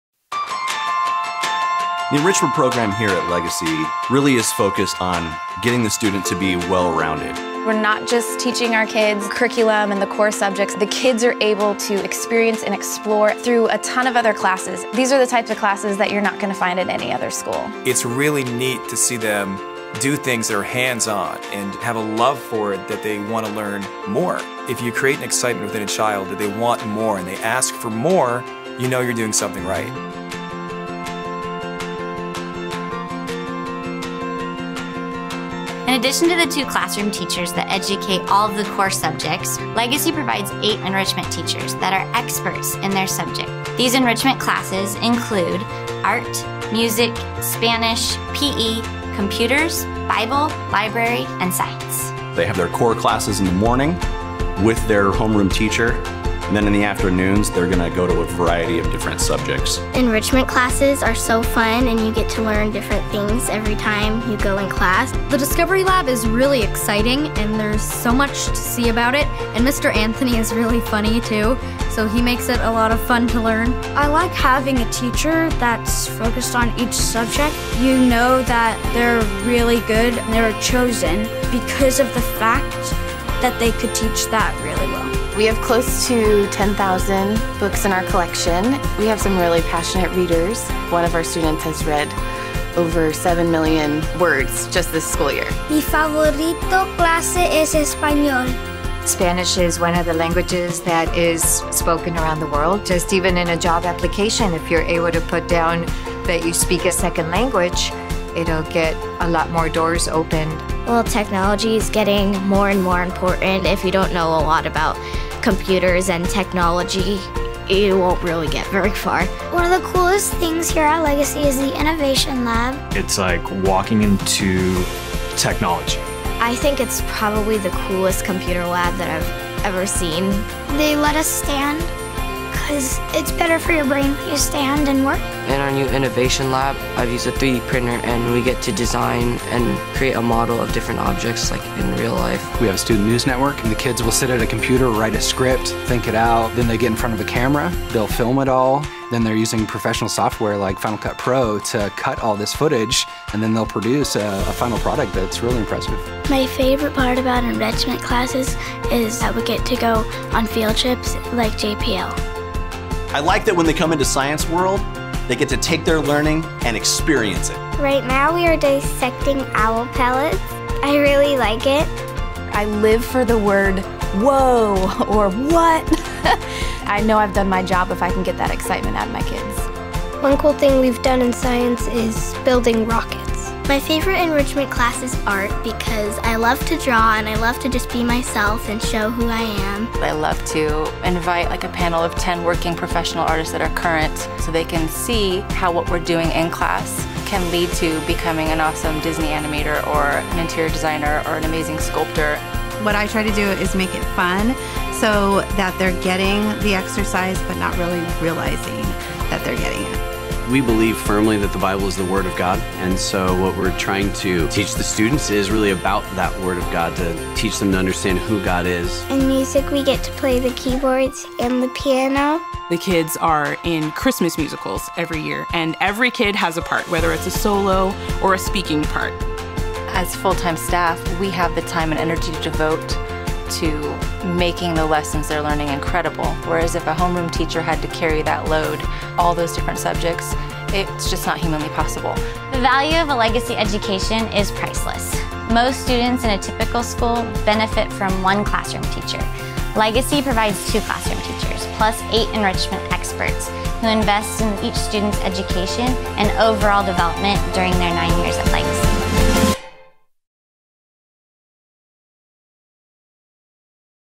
Legacy Christian Academy Enrichment Program
In addition to the two classroom teachers who lead all the core subjects (English, Math, Science, and Social Studies) Legacy provides eight enrichment teachers who are experts in their fields. These enrichment teachers make up the following subjects Art, Music, Spanish, Physical Education, Computers, Bible, Library, and Science. As full time staff these enrichment teachers have the time to create incredible lessons that provide hands-on exploratory opportunities for students.